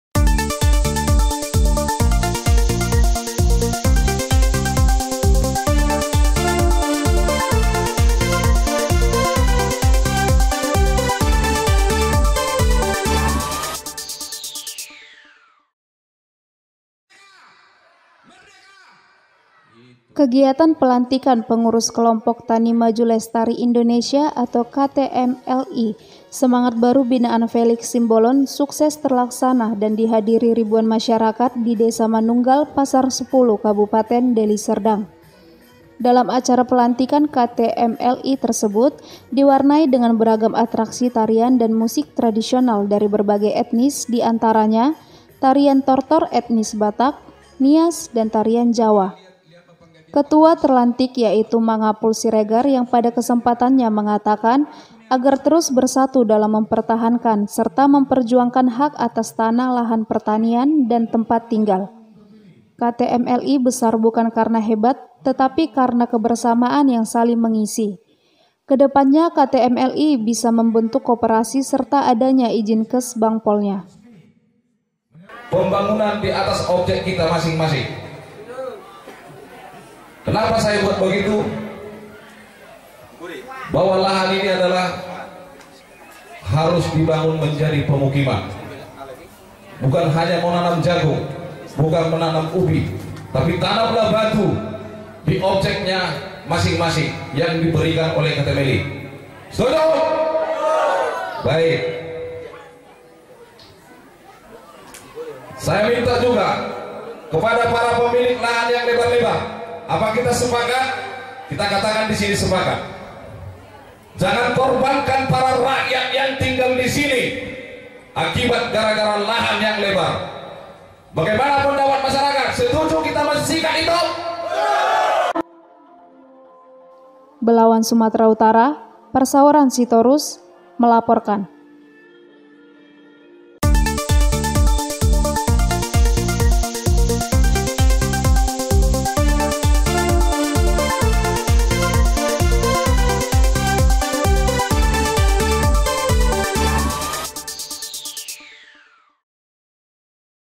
KEGIATAN PELANTIKAN PENGURUS KELOMPOK TANI MAJU LESTARI INDONESIA ATAU KTMLI
Kegiatan pelantikan pengurus kelompok Tani Maju Lestari Indonesia atau KTMLI semangat baru binaan Felix Simbolon sukses terlaksana dan di hadiri ribuan masyarakat di desa manunggal pasar 10 kabupaten deli serdang.

Dalam acara pelantikan ktmli tersebut di warnai dengan beragam atraksi tarian dan musik tradisional dari beragam etnis di antaranya tarian tor-tor etnis Batak, Nias dan tarian jawa.

Ketua yang terlantik yaitu Mangapul Siregar yang pada kesempatannya mengatakan agar terus bersatu dalam mempertahankan serta memperjuangkan hak atas tanah lahan pertanian dan tempat tinggal.

KTMLI besar bukan karena hebat tapi karena kebersamaan yang saling mengisi, kedepannya KTMLI bisa membentuk koperasi serta adanya izin Kesbangpolnya.

Tim Liputan : Parsaoran Sitorus

Salam Tribrata.